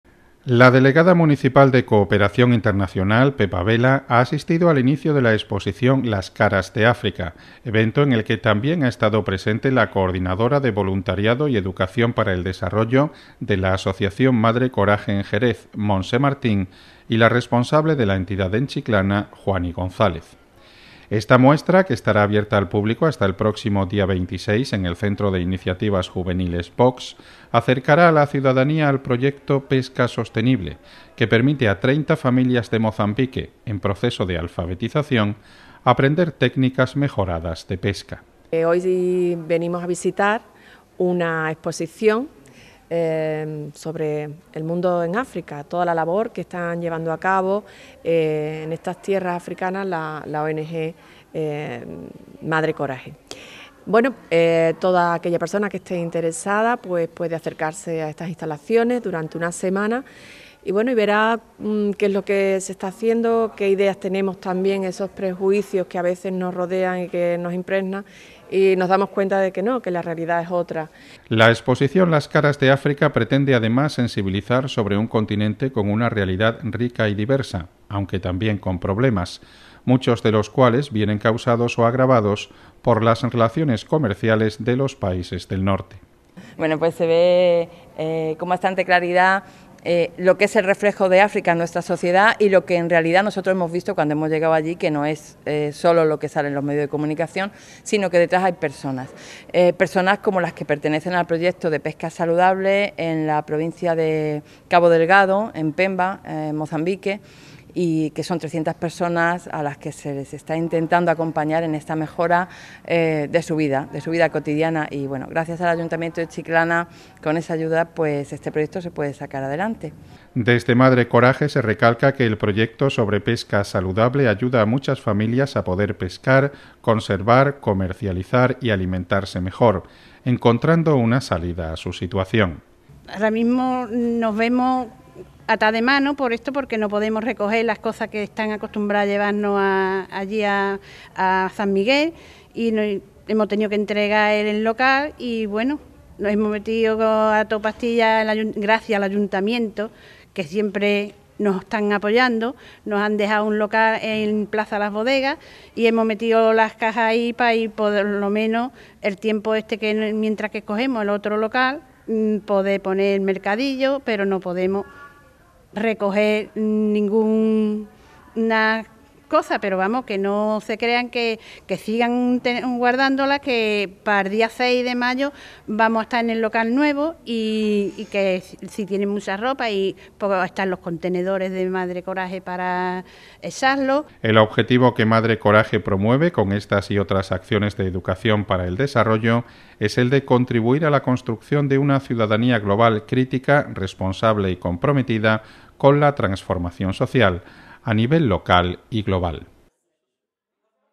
INAUGURACIÓN EXPOSICIÓN "CARAS DE ÁFRICA"
El Centro de Iniciativas Juveniles Box acoge la exposición “Las caras de África” hasta el martes 26. Esta muestra, organizada por el Ayuntamiento y Madre Coraje, acercará a la ciudadanía al proyecto ‘Pesca Sostenible’, que permite a 30 familias de Mozambique, en proceso de alfabetización, aprender técnicas mejoradas de pesca.